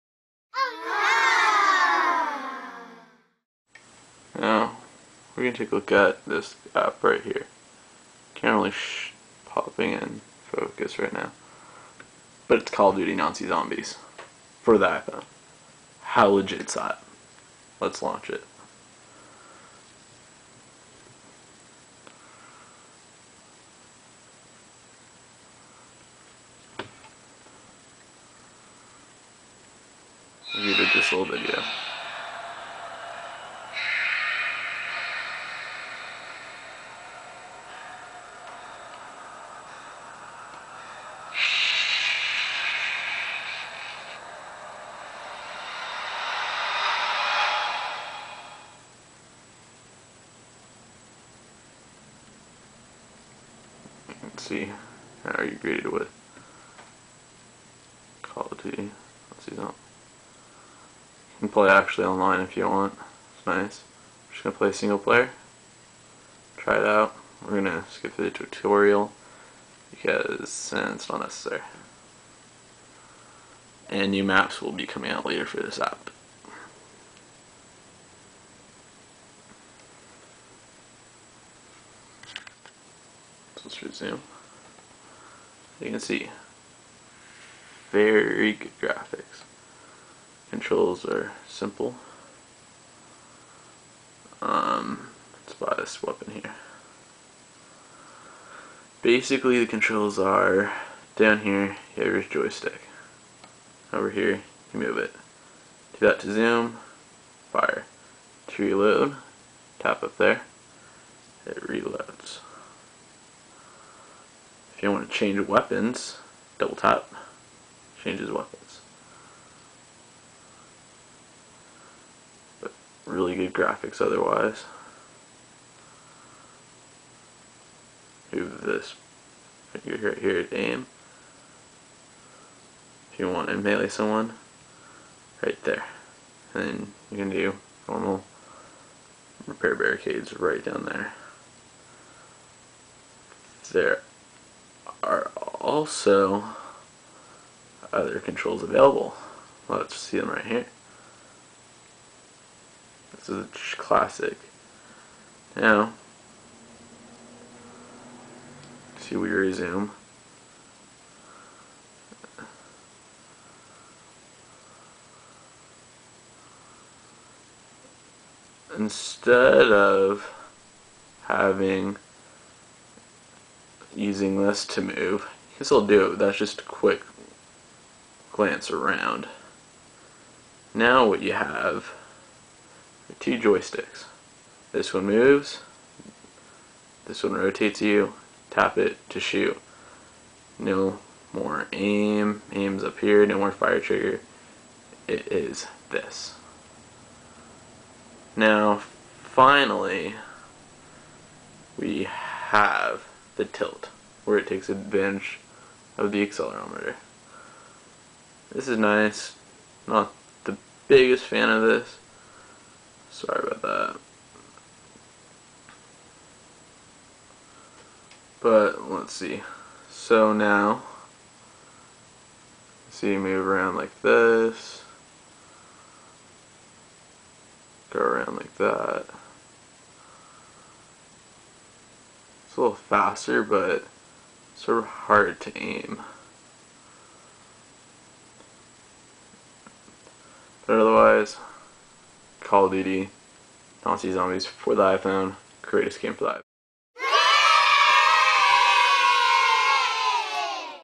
Call of Duty Nazi Zombies iPhone App
Here is an App review of Call of Duty Nazi Zombies. Let me just say that this is an amazing app. I highly recommend it. for more reviews.